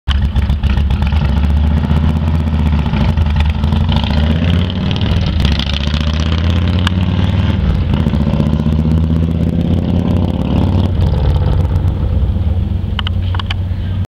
2006 Yamaha Road Star.AVI
This is a 2006 Yamaha Road Star that we are selling on our ebay. To check it out or any of the other cars trucks and motorcycles that we are selling go